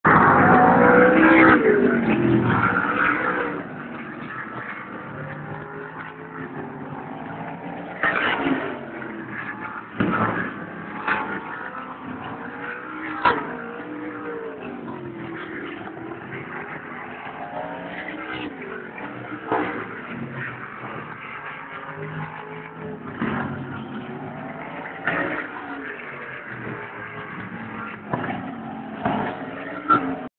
MSA-Oswego Non Wing Challenge @Sandusky Speedway 09/18/10 Practice
Wing Side Up Enterprises video of the MSA/Oswego Non-Wing Challenge Practice at Sandusky Speedway. Held 09.18.10.

To learn more about this event and "The Ultimate Short Track Race Car" be sure where it's "All Supermodifieds, All of the Time!"Sent via BlackBerry from T-Mobile